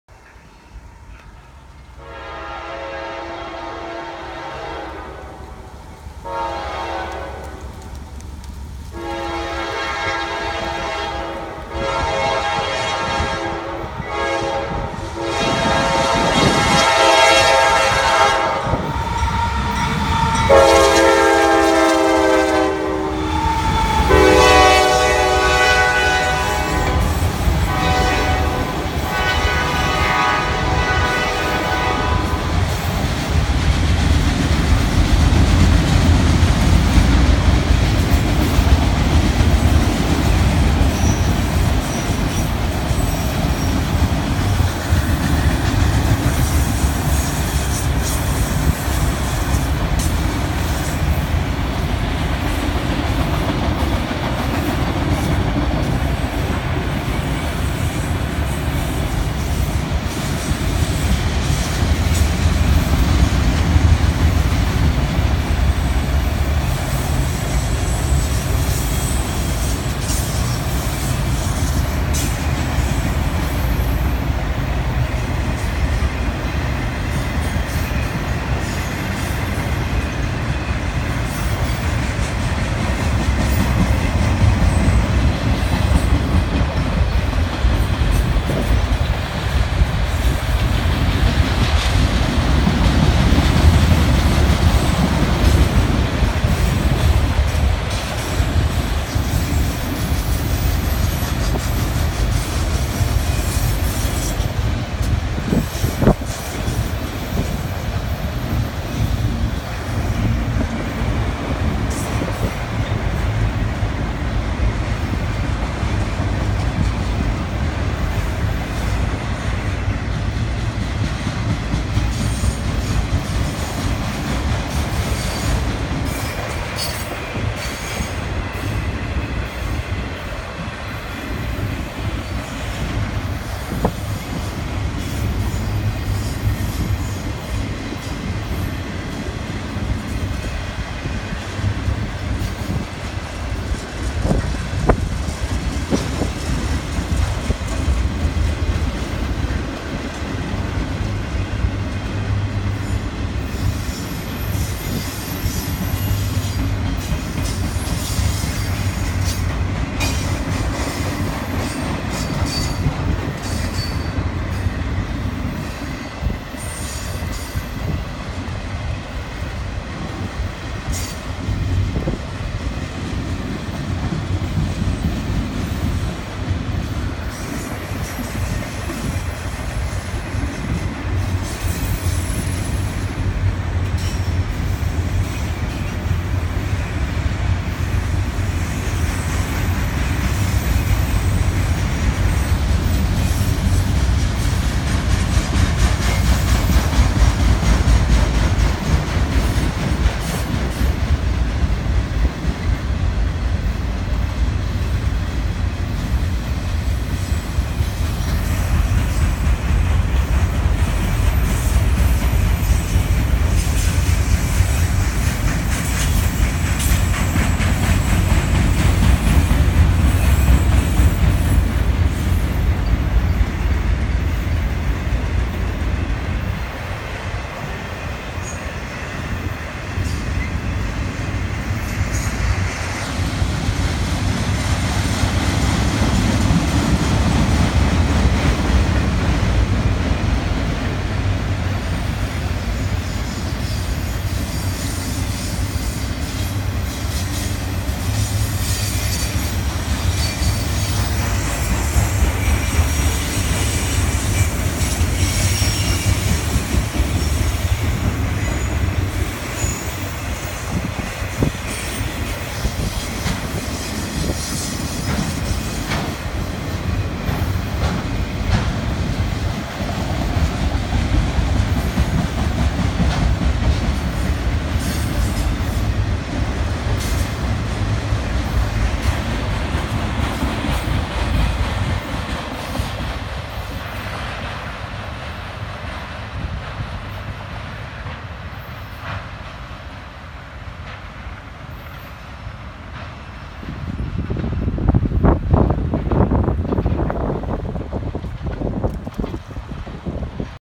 CSX Auto Rack Freight train @ Folkston, GA 2/11/18 in 4K
A CSX Frieght Train passing through Folkston, GA on 2/11/18.